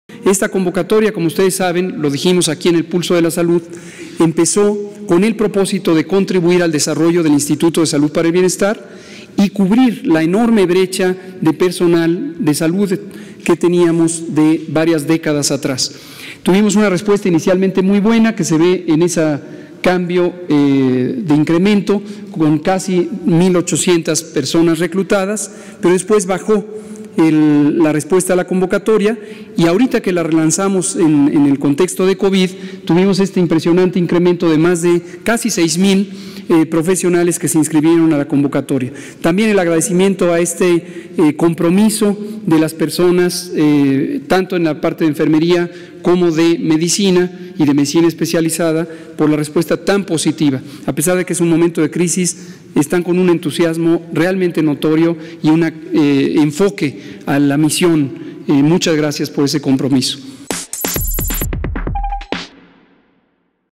75, 927 registros totales durante la Jornada Nacional de Reclutamiento del sector Salud
«Durante la Jornada Nacional de Reclutamiento del sector Salud México contra el COVID19 se consignaron 75, 927 registros totales», dijo el subsecretario de Salud, Hugo López-Gatell.